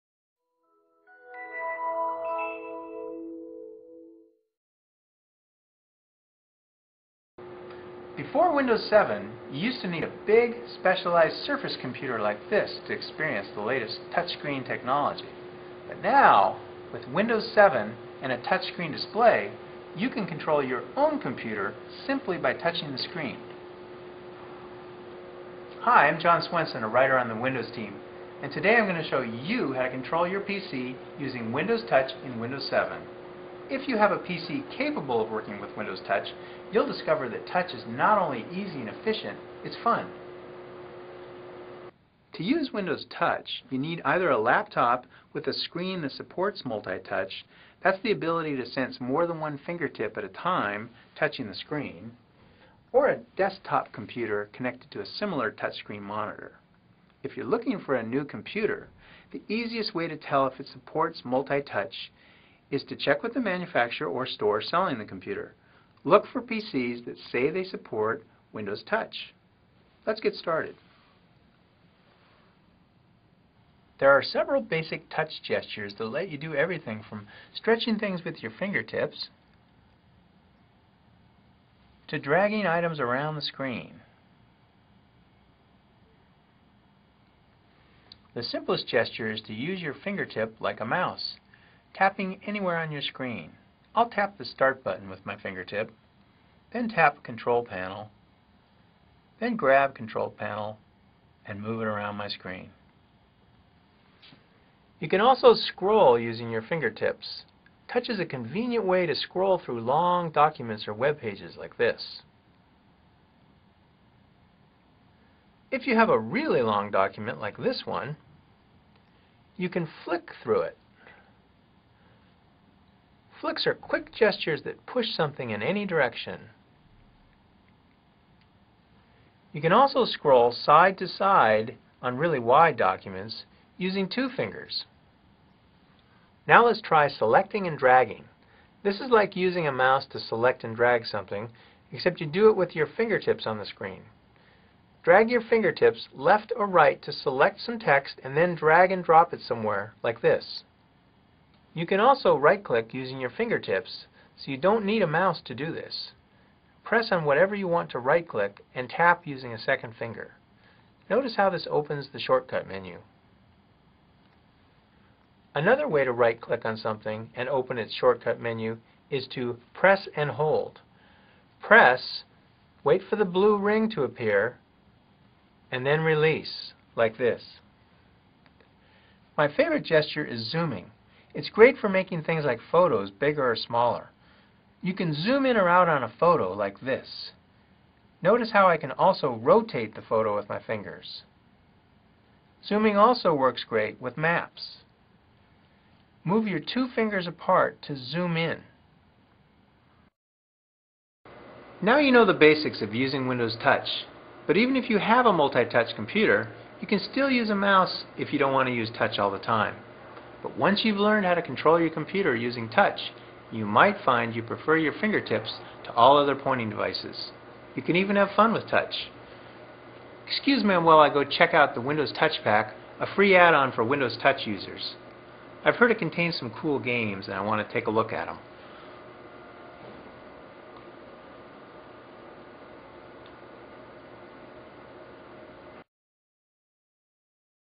Using Windows Touch
This video shows you how to interact with your PC using Windows Touch in Windows 7. (3:55)